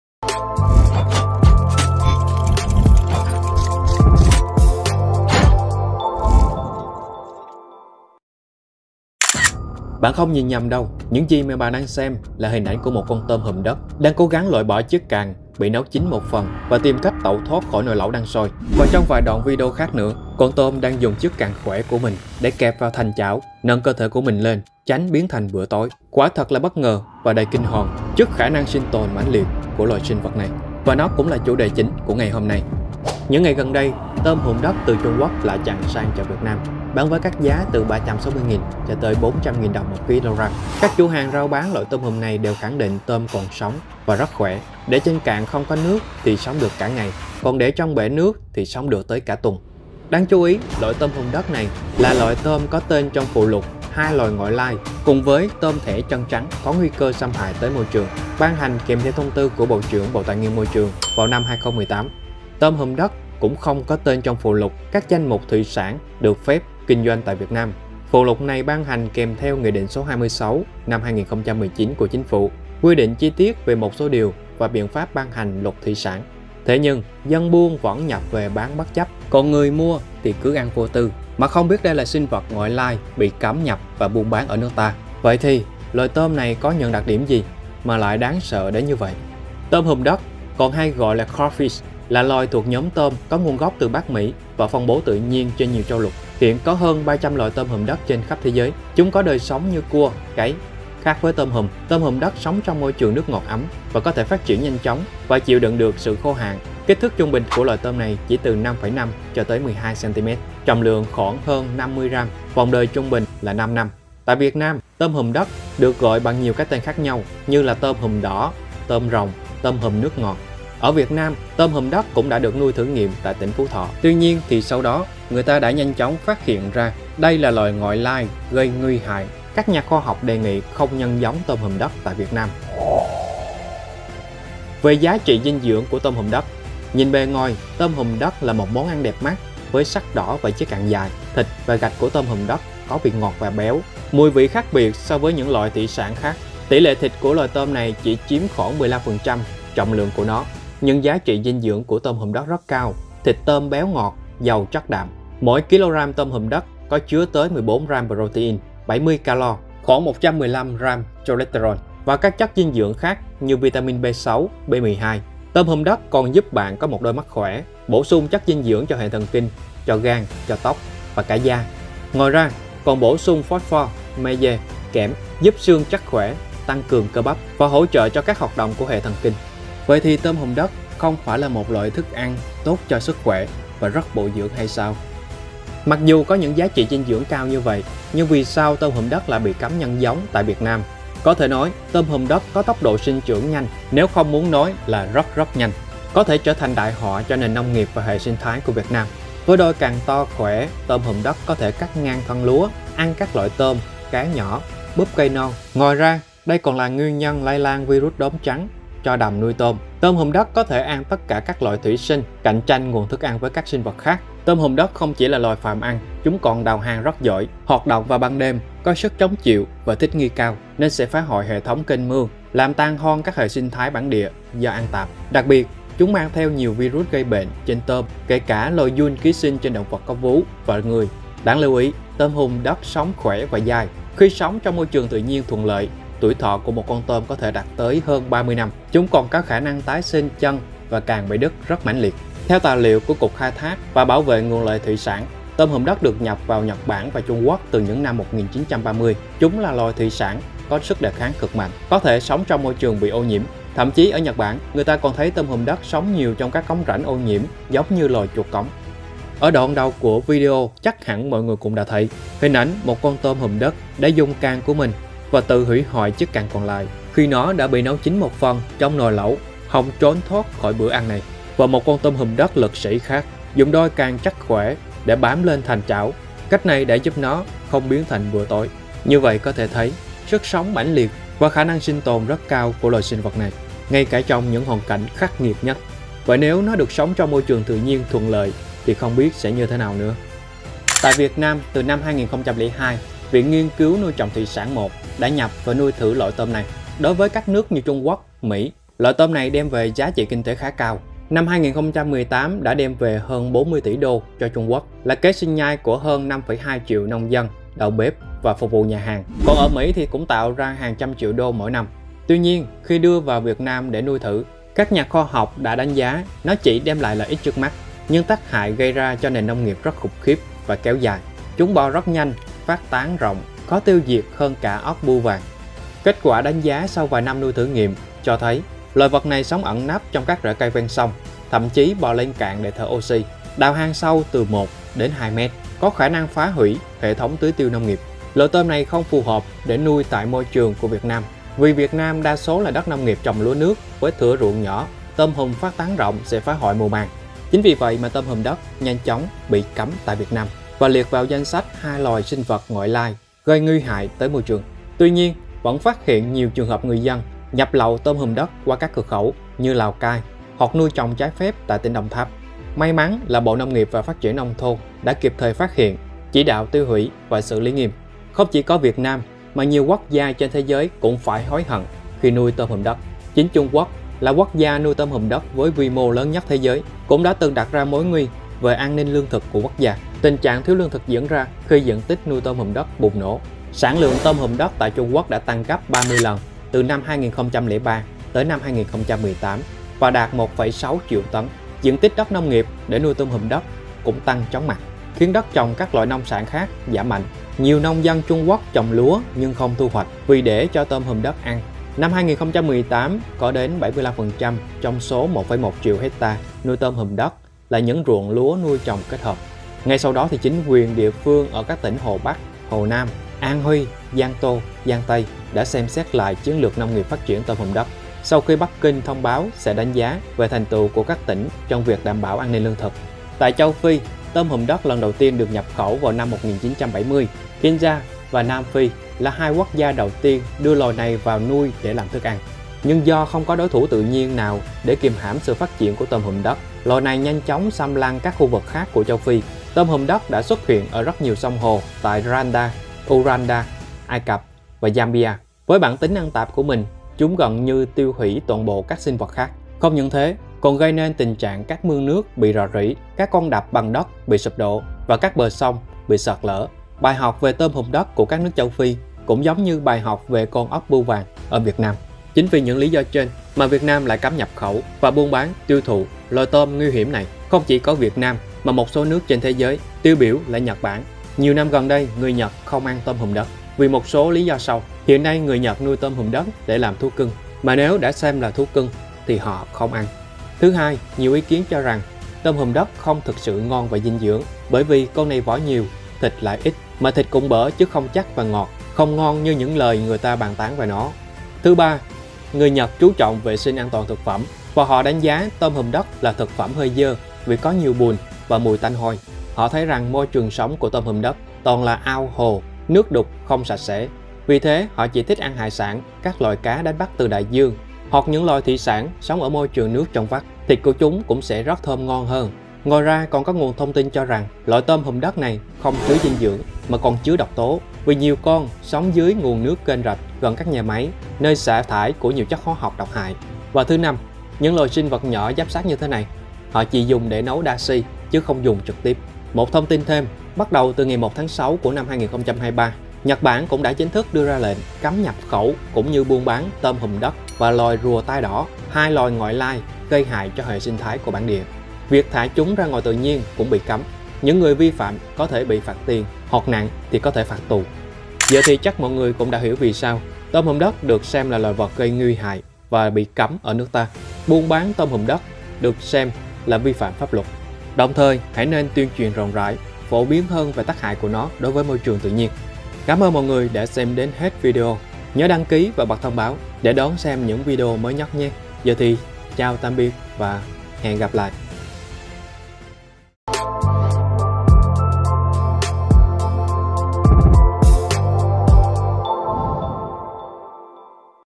TÔM HÙM ĐẤT - NHẬT BẢN KHÔNG ĂN, VIỆT NAM DÙ CẤM NHƯNG VẪN LÉN BÁN | DCT
Tôm hùm đất (tên khoa học Procambarus clarkii) nuôi ở nhiều tỉnh của Trung Quốc. Nikkei (Nhật Bản) cho hay loài này mang lại giá trị kinh tế gần 40 tỷ USD cho Trung Quốc trong năm 2018, là sinh kế của hơn 5,2 triệu nông dân, đầu bếp và phục vụ nhà hàng. Ở Mỹ, tôm hùm đất cũng tạo ra giá trị hàng trăm triệu USD mỗi năm.

Gần đây, loài TÔM HÙM ĐẤT này được bán nhiều tại Việt Nam. Dù vậy, việc nuôi, kinh doanh tôm hùm đất lại bị cấm do vi phạm quy định về đa dạng sinh học và thủy sản.

Lý giải "vì sao tôm hùm đất đã chứng minh được hiệu quả kinh tế ở Mỹ, Trung Quốc mà Việt Nam lại cấm?". Loài TÔM HÙM ĐẤT này đưa vào nuôi ở Việt Nam có thể đem lại chút lợi ích trước mắt, nhưng tác hại gây ra cho nông nghiệp rất khủng khiếp, kéo dài. Chúng bò nhanh, phát tán rộng, khó tiêu diệt hơn ốc bươu vàng.
Trung Quốc đang phải gánh chịu những hậu quả không hề nhỏ về môi trường sinh thái do tôm hùm đất gây ra, điển hình là việc chúng phá hoại ở dọc sông Trường Giang. Môi trường nông nghiệp Các nước Châu Phi cũng từng bị phá hủy nặng nền bởi loài TÔM HÙM ĐẤT này.

"Việt Nam sở hữu nhiều loài thủy sản có thể tập trung phát triển thành nguồn lợi lớn, tại sao phải chọn nuôi một loài mới từ nước ngoài khi biết nó đem lại quá nhiều rủi ro cho nông dân?", ông Hùng nêu quan điểm và cho rằng "không thể thấy nước khác nuôi con gì mình cũng chạy theo". Trước khi đưa loài mới vào nuôi trồng, cơ quan quản lý phải cân nhắc kỹ càng yếu tố kinh tế, môi trường.

Nhiều năm trở lại đây, người dân Nhật bản không ăn TÔM HÙM ĐẤT nữa vì một số lý do. Không chỉ có Việt Nam mà chính Nhật Bản cũng chính thức ban hành chính sách Cấm buôn bán Tôm Hùm Đất và Rùa Tai đỏ, được công bố là hai loài ngoại lai nguy hại cho môi trường bản địa. Thâm chí, nếu vi phạm còn bị phạt tiền hoặc nặng có thể phạt tù.

► Thêm 1 nụ cười, thêm 1 chút thộng tin.